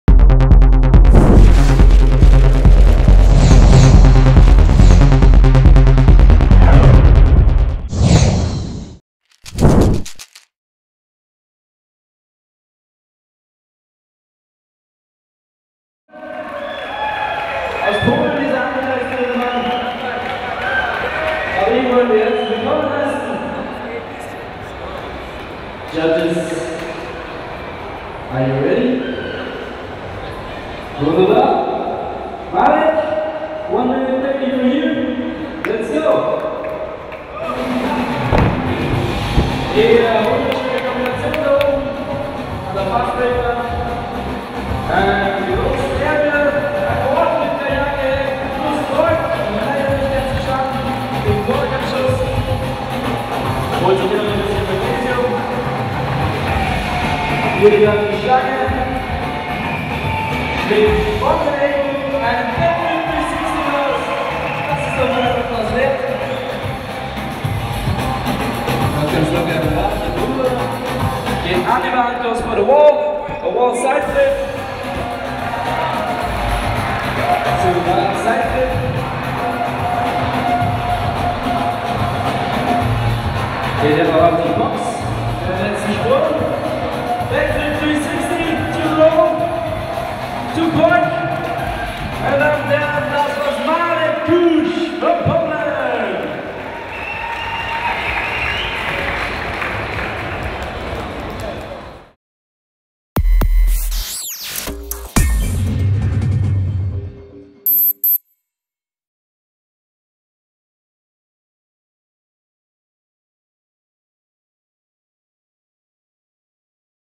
PCWC2011 | Marek Kuś -- Poland | 9th Style Contest
Marek Kuś at the Style Contest of FASTBREAK PARCOURING™ WORLD CHAMPIONSHIP 2011 in Berlin.